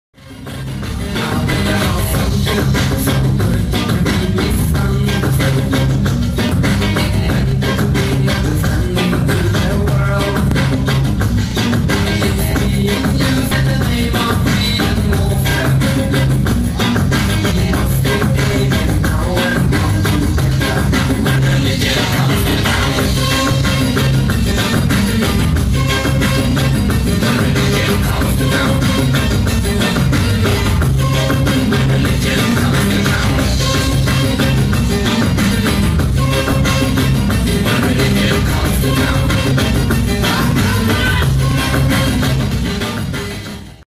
Dance-show in SIRIA -arabian dance-(2006)
Dancers from Belarus -MIKHAILAVA NATALLIA,...